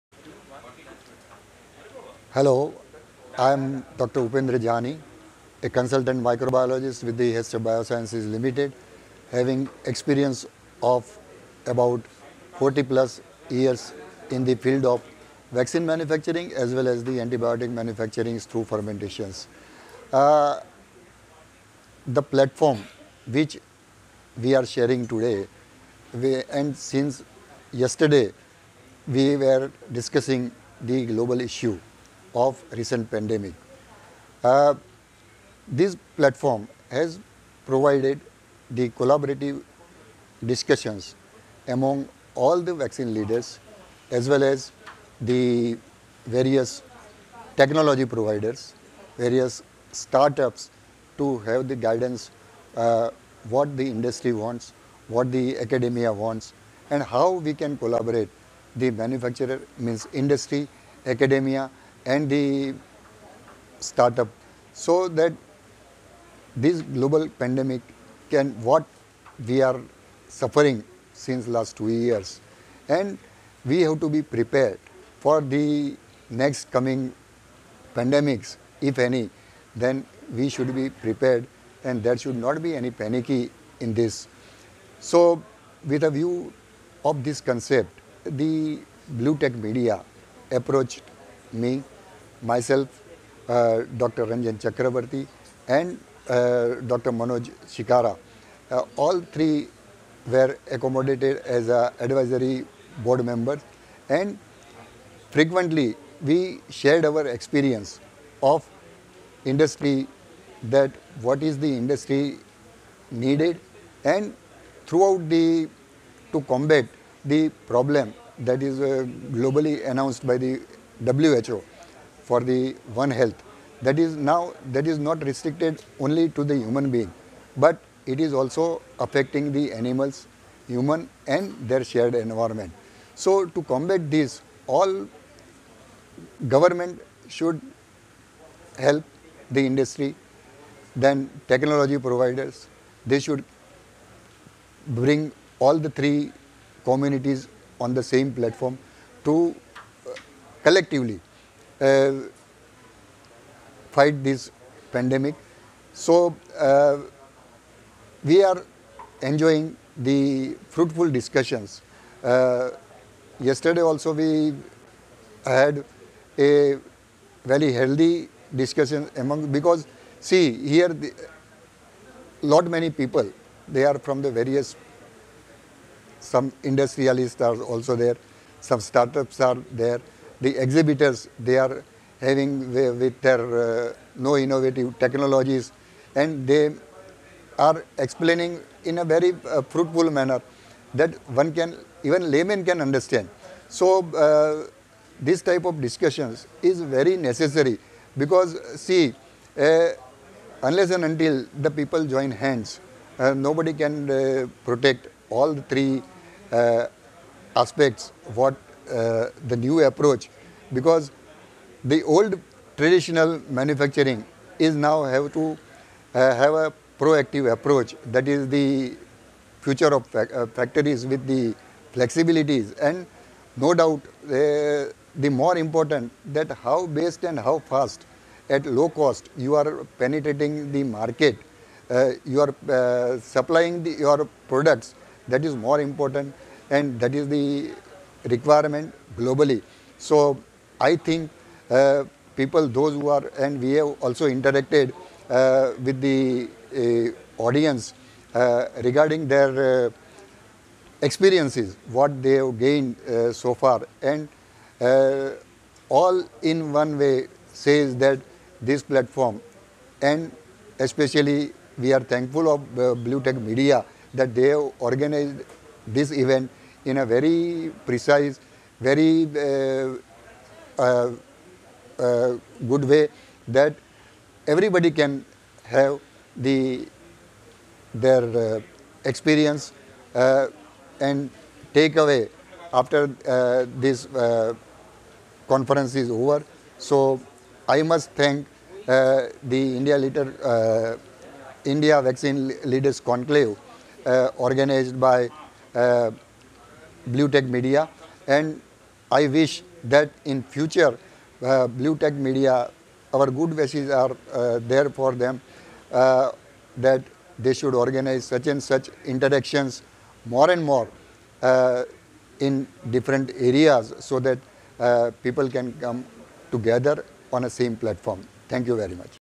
Dr Upendra Jani, Consultant Microbiologist, Hester Biosciences Limited | IVLC 25th-26th Aug 2022
India Vaccine Leaders Conclave
25th-26th Aug 2022, Mumbai